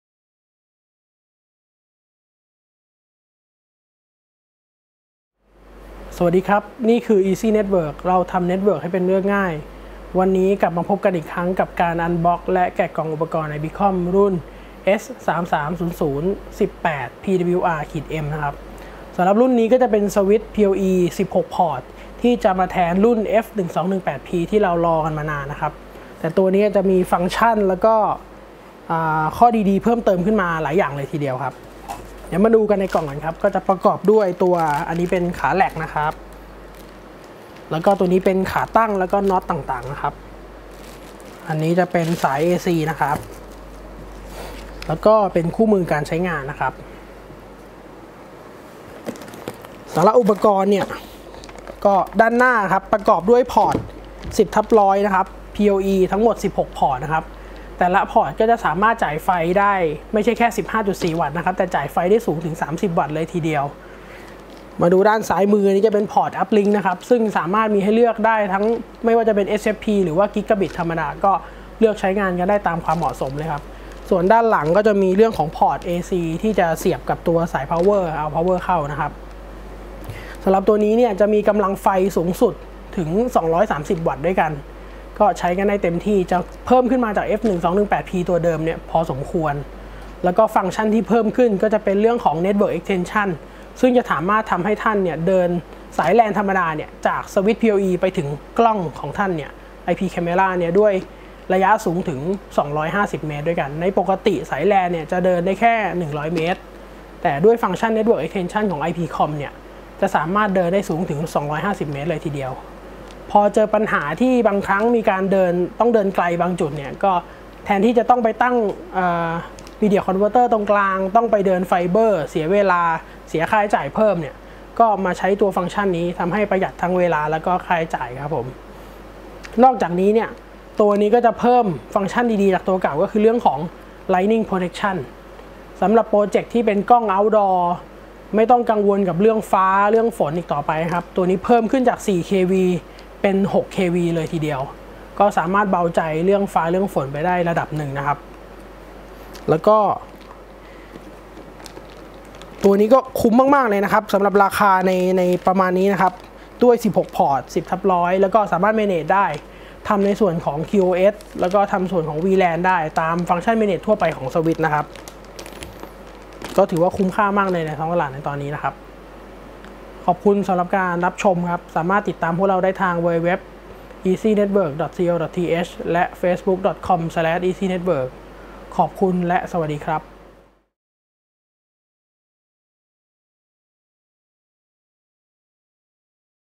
IP-COM : PoE Switch S3300-18-PWR-M - Unbox แกะกล่องพรีวิวสินค้า
วันนี้เราจะมาพรีวิวสินค้า อุปกรณ์ PoE Switch รุ่น S3300-18-PWR-M
ซึ่งเป็น Switch 16 Port มี function ต่างๆ และ ข้อดีเพิ่มขึ้นมากเลยทีเดียว !!

สามารถแก้ปัญหาต่างๆ ได้อย่างง่ายดาย
ประหยัดเวลา และค่าใช้จ่ายขึ้นแน่นอน

เหมะสมกับ งานที่เป็น Outdoor สำหรับกล้อง CCTV ด้วย Function ที่สะดวก และ รวดเร็วขึ้น เรียกได้ว่า คุ้มค่าเหมือนเดิม
รับประกันถึง 2 ปี !!

รายละเอียดเพิ่มเติมที่